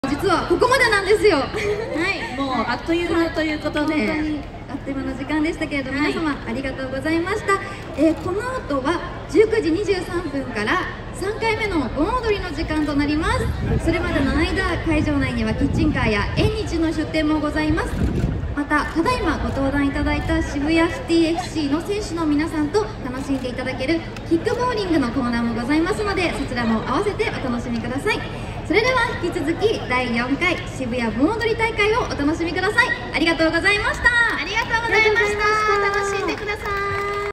今後のスケジュールを告知するAKB48（中西智代梨、岩立沙穂、千葉恵里）
2023年8月5日　第4回渋谷盆踊り